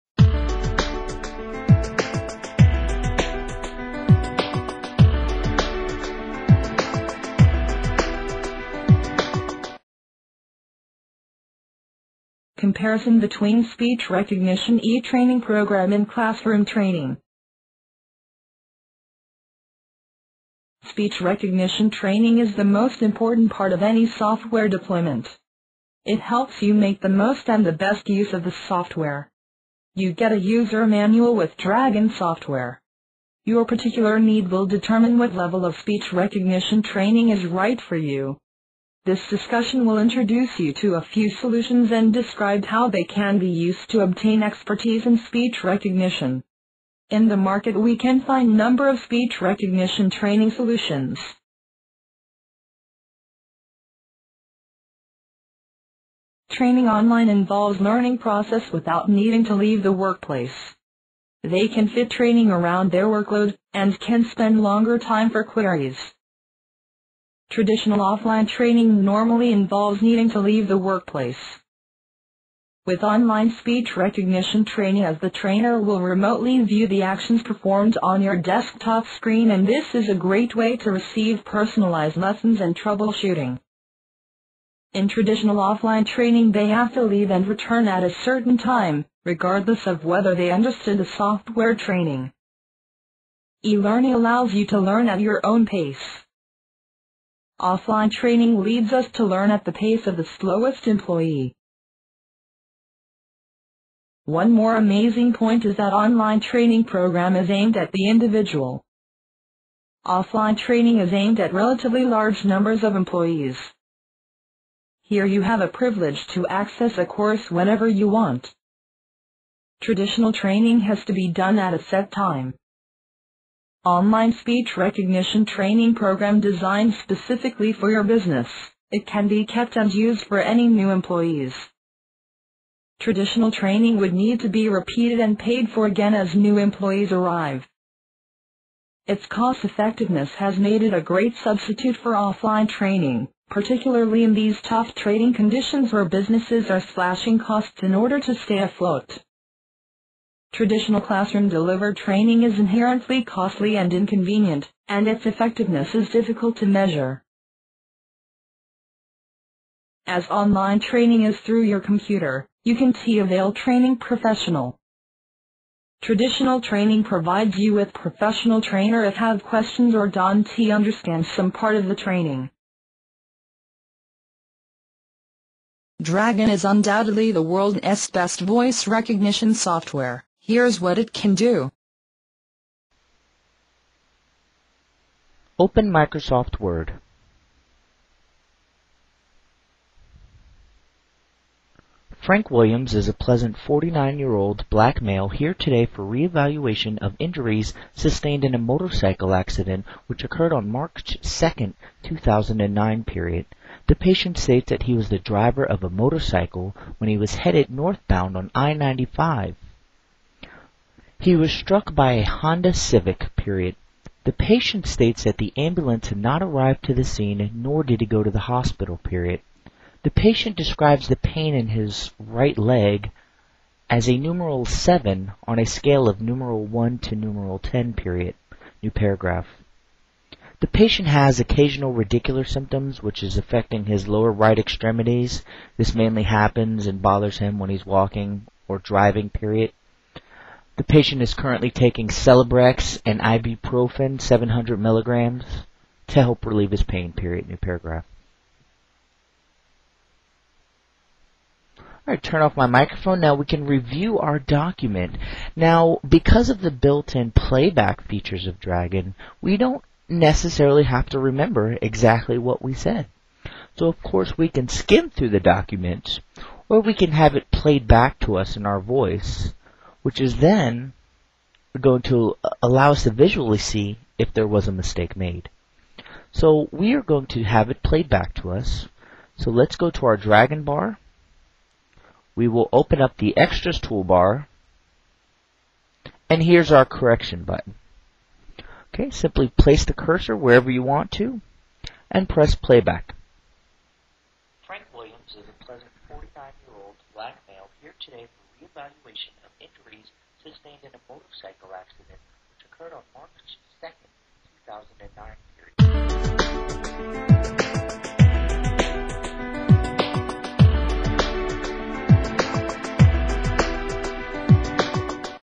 E-training vs Class room training for Speech Recognition
This video represents the difference between online training and classroom training in the specific vertical of Speech Recognition.